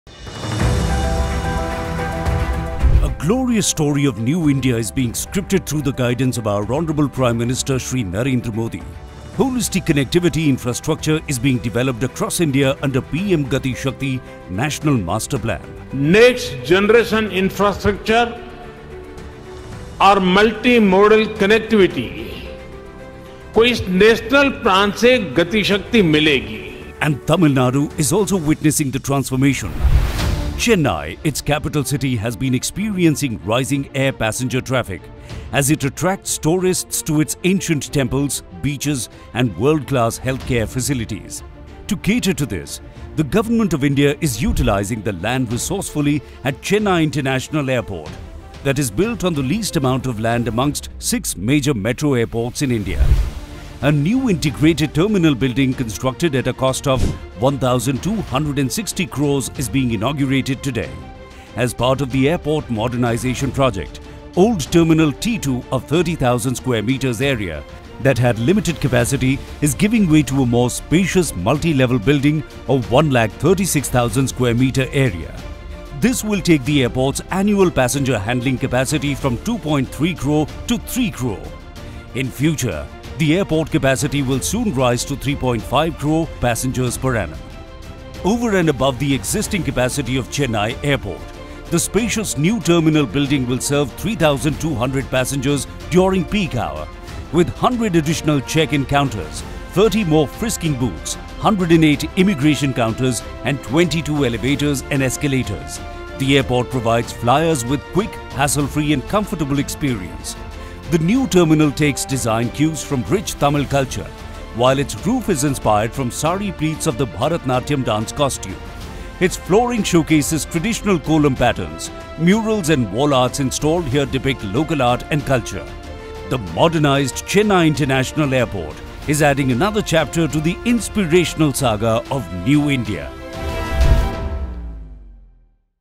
New terminal building of Chennai airport: Modern marvel of aviation infrastructure
Prime Minister Narendra Modi inaugurated the New Integrated Terminal Building (Phase- 1) of Chennai International Airport, developed at the cost of Rs. 1260 Crore. The addition of this new Integrated terminal building will increase the passenger serving capacity of the airport from 23 Million Passengers Per Annum (MPPA) to 30 MPPA. The new terminal is a striking reflection of the local Tamil culture, incorporating traditional features such as the Kolam, Saree, Temples, and other elements that highlight the natural surroundings.